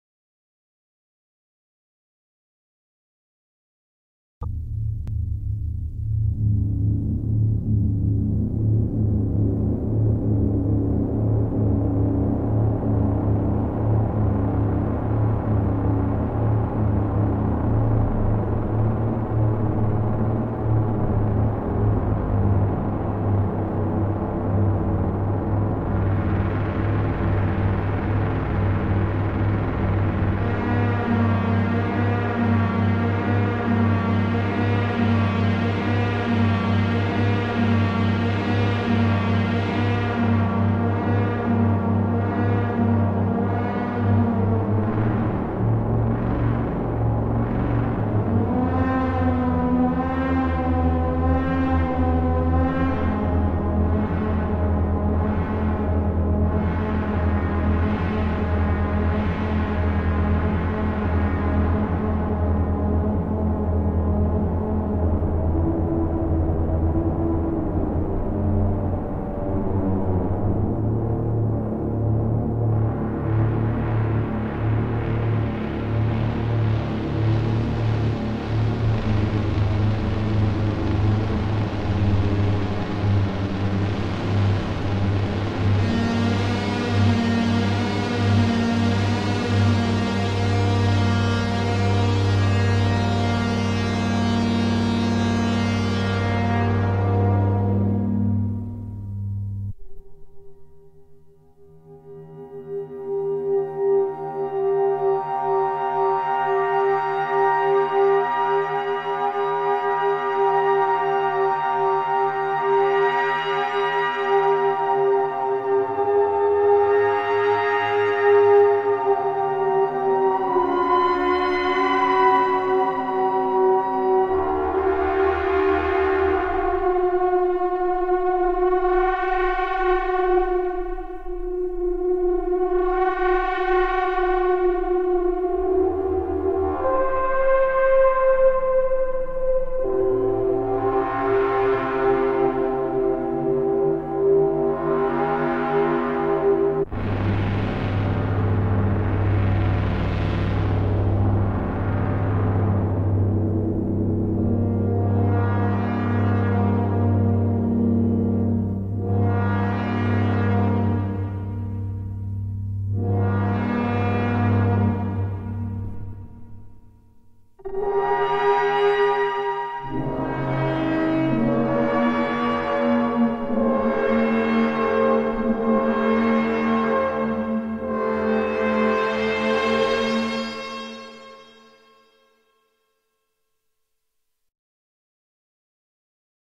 Trying to make a Liven XFM sound like a Soma Lyra-8
Made January 2021;

I was inspired by people on youtube jamming on the Soma Lyra-8 recently. But I don't have one. So I tried to create a spacey horror drone with just the Sonicware Liven XFM, no external effects or processing, just internal reverb, recorded raw into Cubasis 2.

Realistically, a $200 digital FM groovebox isn't really gonna sound like a $1200 premium 8 voice poly analog, but it's fun to try!

So this was really more a challenge to get creative, and to have an excuse to make dystopian atonal drones and force you to listen to it.